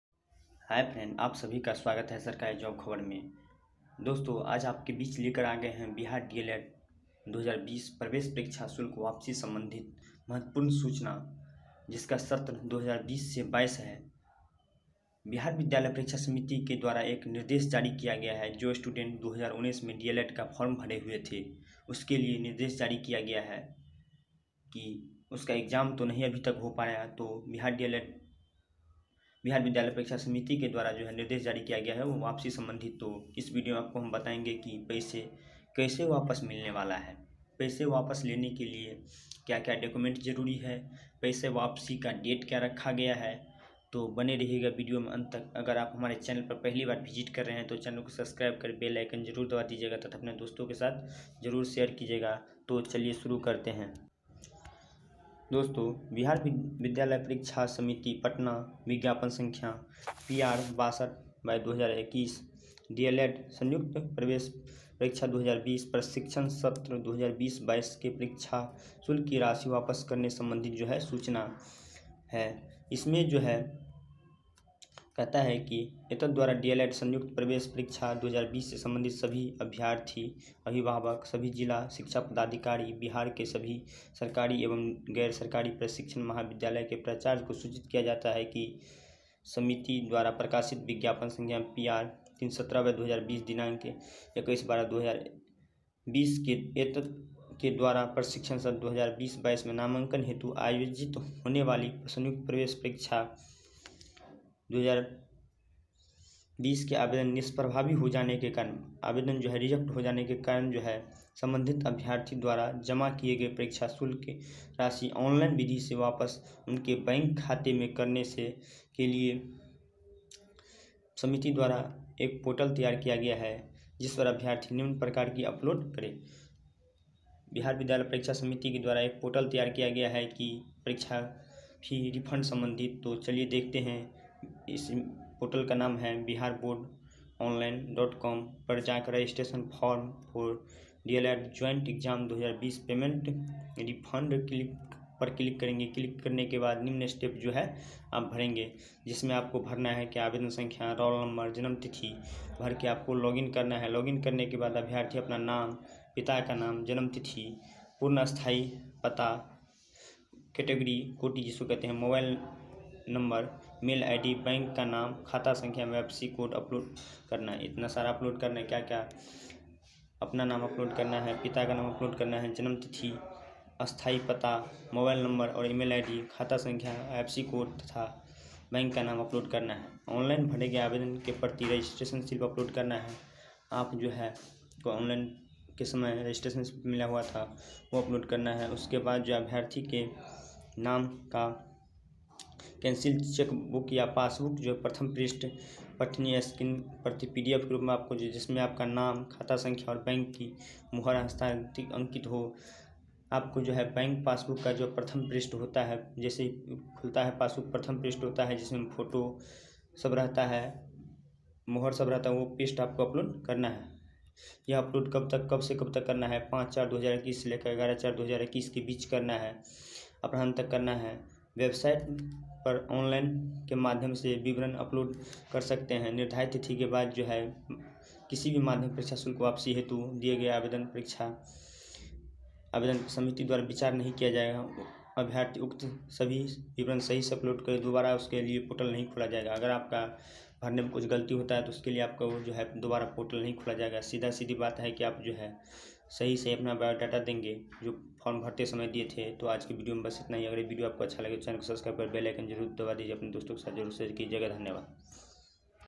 बिहार D.El.Ed :-2020 प्रवेश परीक्षा शुल्क वापसी संबंधित महत्वपूर्ण सूचना/ सत्र:-2020-22/सभी छात्र के
Telegram channel sarkari job khabar